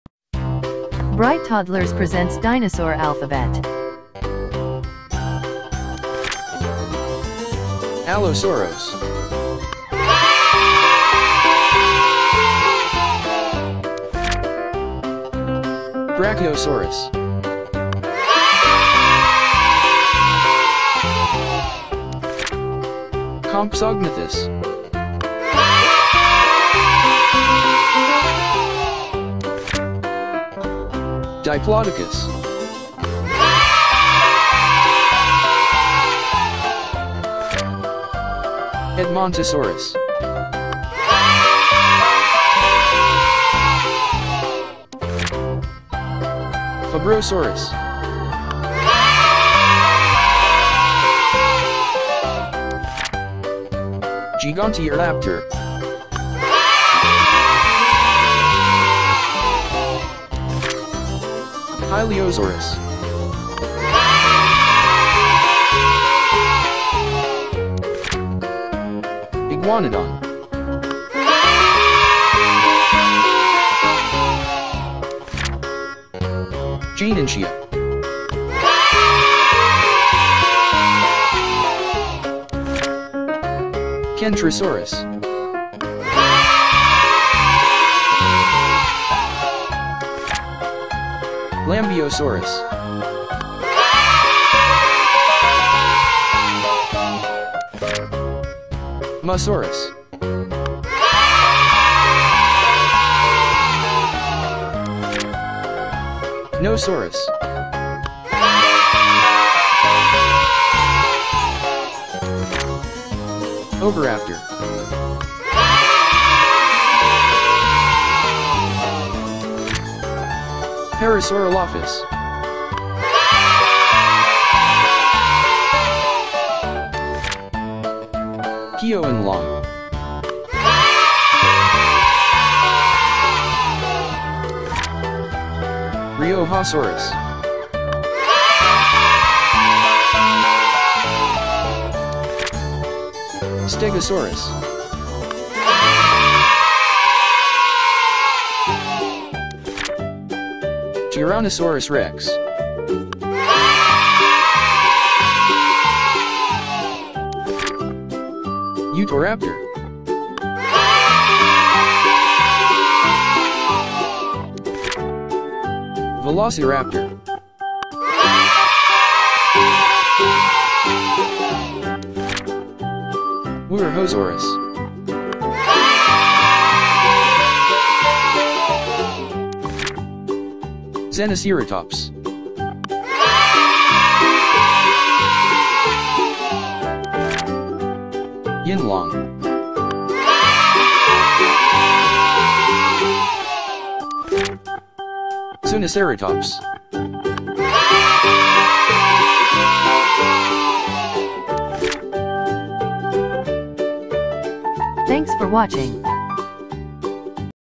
Learn Dinosaur for kids | Dinosaur Flash Cards for Children | Educational videos for Toddlers
In this video, kids will learn about dinosaurs.
Includes Tyrannosaurus rex, Stegosaurus, Velociraptor, Brachiosaurus, Iguanodon, Diplodocus, Parasaurolophus and many more...